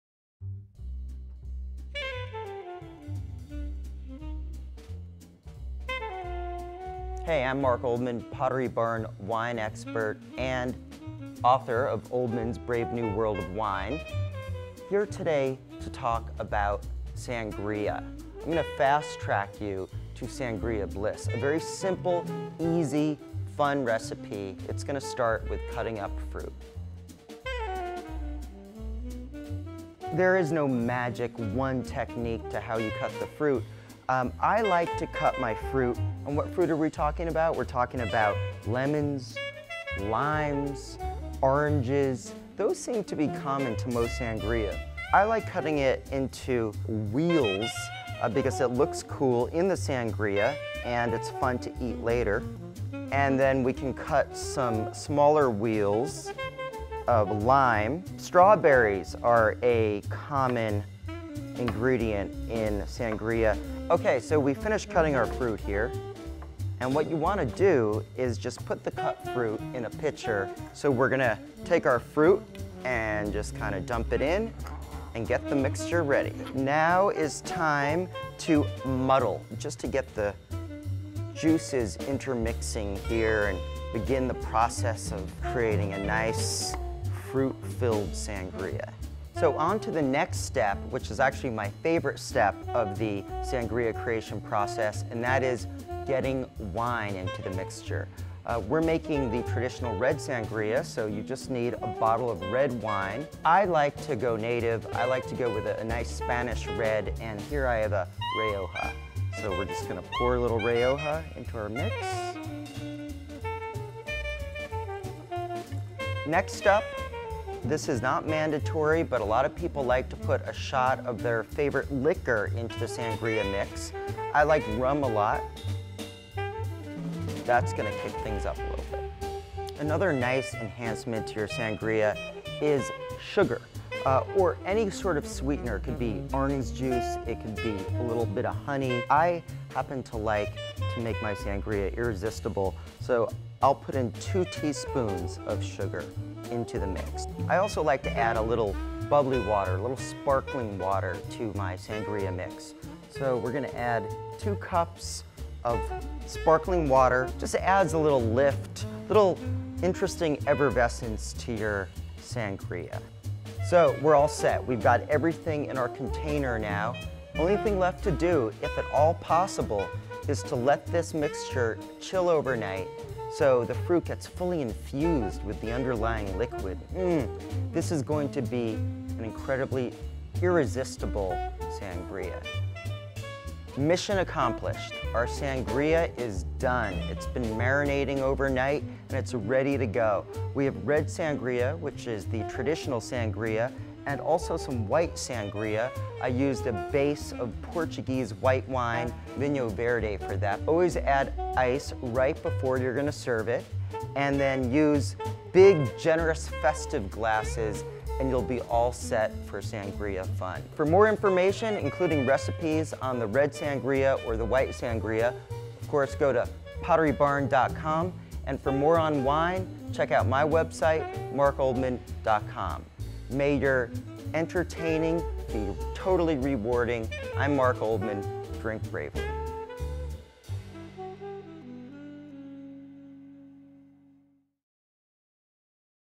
How to Make Traditional Red Sangria with Mark Oldman | Pottery Barn
Pottery Barn wine expert and author of "Oldman's Brave New World of Wine", Mark Oldman, shows how to make sangria in this video. For this traditional red sangria recipe, first cut up fruits like strawberries, lemons, lemons, and oranges. Cut the fruit into small wheels since they look good and are fun to eat later. Put the cut fruit in a sangria pitcher. Press the fruit and mix it.

Add red wine such as a Spanish red wine to the fruit mix. Add a shot of your favorite liquor, for example rum, to the mixture. Next, add sugar or honey to this sangria recipe. Then add two cups of sparkling water. Let this mixture chill overnight. This red sangria, a traditional sangria, is now ready. Now that you know how to make sangria, serve it with ice in festive glasses and entertain in style!